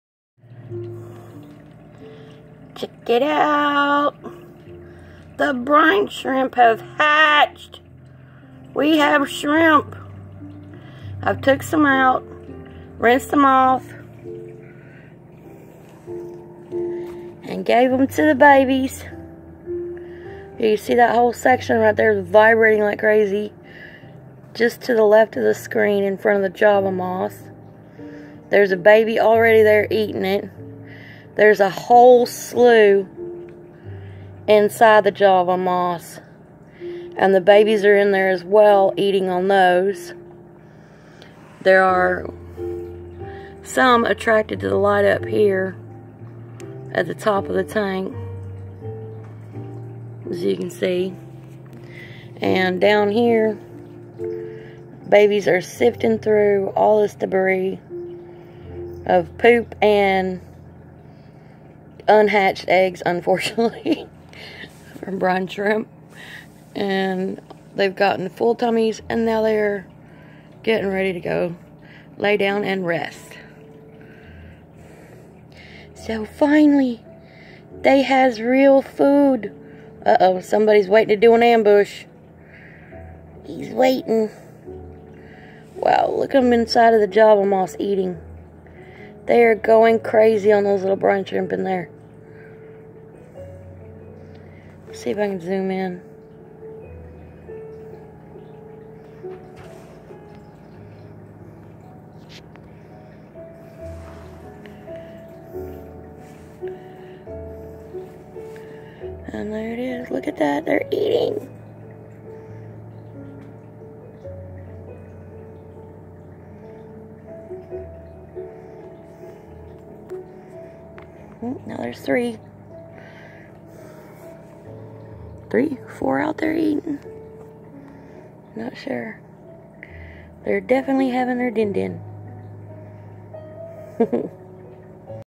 Baby Coradoras First Real Meal of fresh hatched brine Shrimp
After taking 17 hours the Brine Shrimp finally hatched. And Our Baby Coradoras have finally had their first real Meal.
I have been concerned for 4 of the babies today. They were looking a bit sickly and their bellies were still empty. Most of the other babies had started to eat finally crushed shrimp pellet and had visible food in the tummies.  but, these 4 did not.
Now 2 of them have eaten a bit of shrimp and seem to be doing better. But the other 2 are still not as active or feeding yet.  I will keep my eyes  on them and hopefully they will all make it.

For now Fingers Crossed and  have Fun.